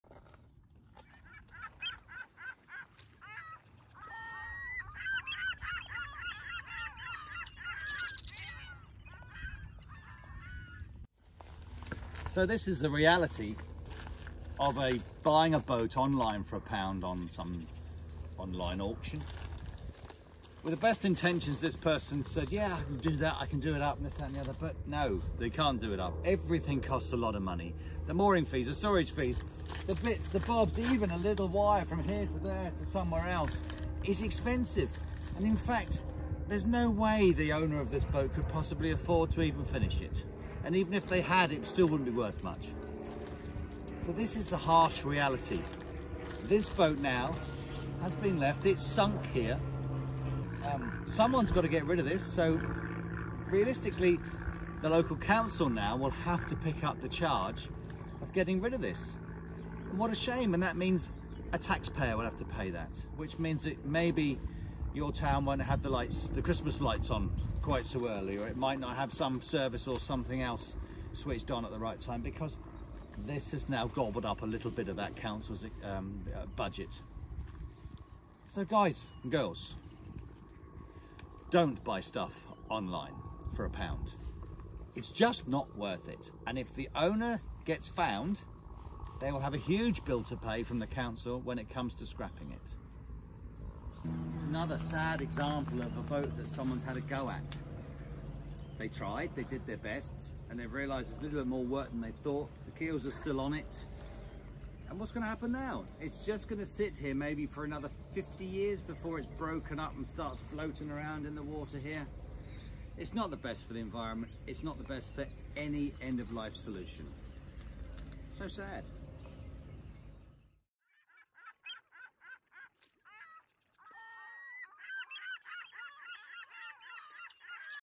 Discussing Abandoned Project Boats in Gosport
The problem of abandoned boats is one that is growing across the UK and Europe. One of the main feeders into this problem is the buying of cheap project boats. These boats are often bought and end up dumped because they have no perceived value to the owners.

In most cases the owners may not have the money to complete the project. Then they definitely don't have the money to scrap the boat through the correct channels. As the boat is left to rot it will begin to break down and harmful GRP microfibres will enter the surrounding environment.

Also if boats end up sinking the bill to collect and dispose of them shoots up as dive teams are so costly to deploy.

In this video Steve talks about the issue near some of the abandoned and soon to be abandoned boats in our local area of Gosport.